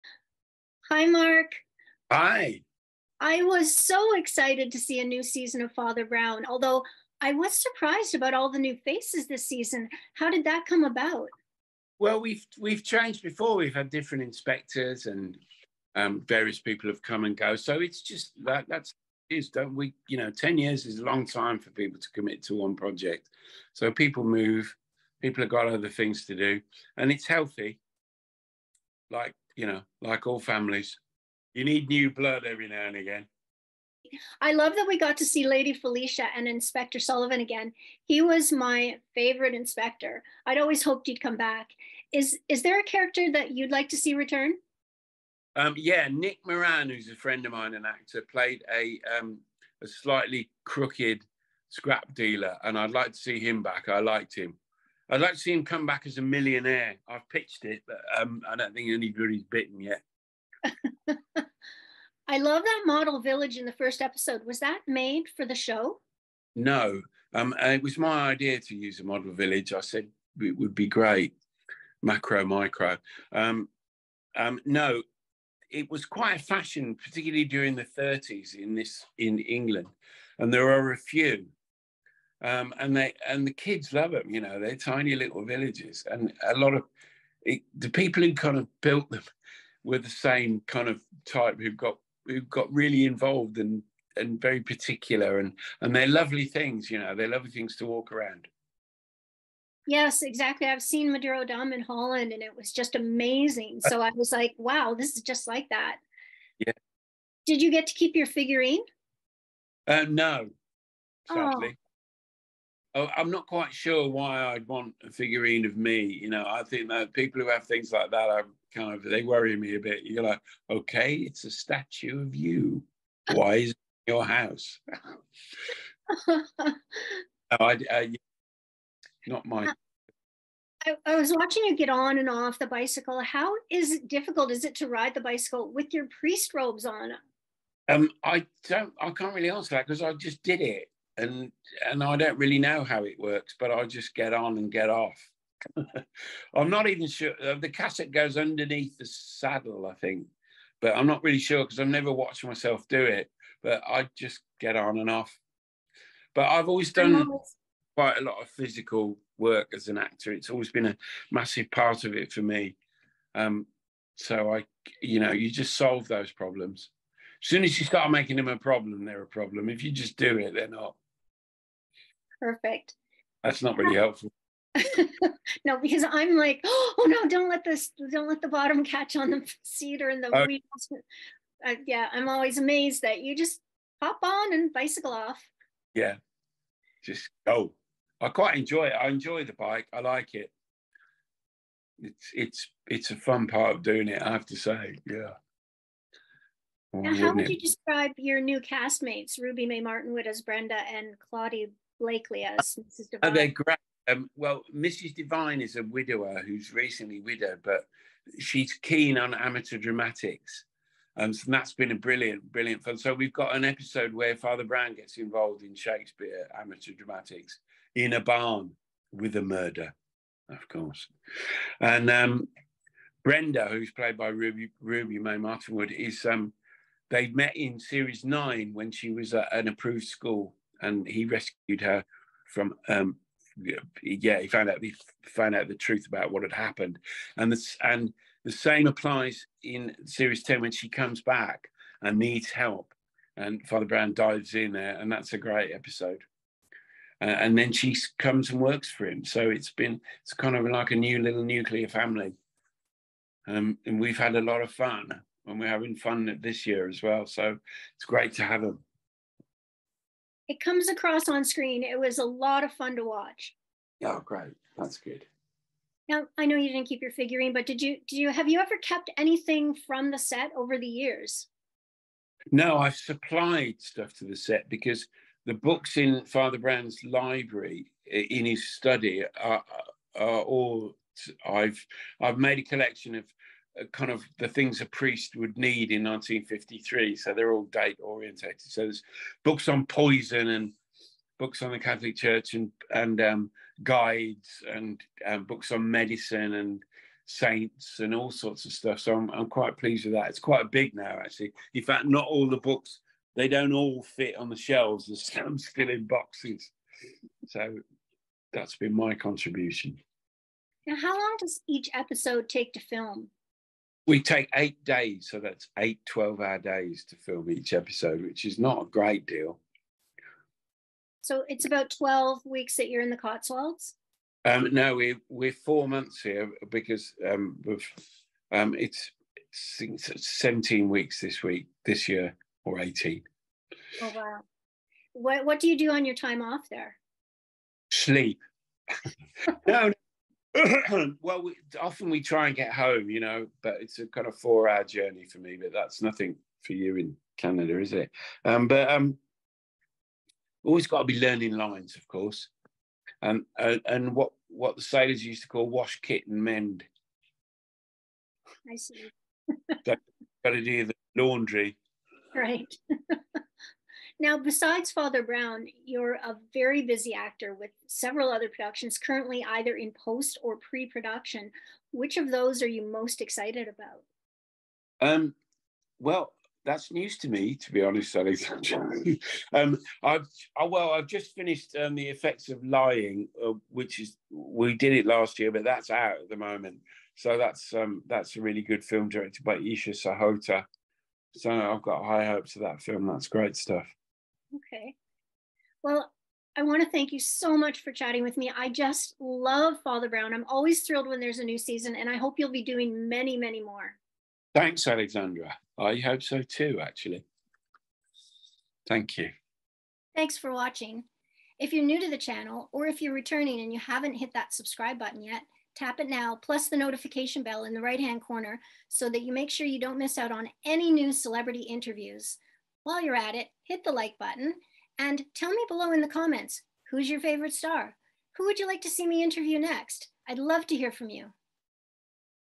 Mark Williams on S10 of Father Brown, cast changes, riding a bicycle while wearing a cassock & more!
Hey Alexandra! - Celebrity Interviews and more!
Alexandra is also an award-winning editor and the author of two books.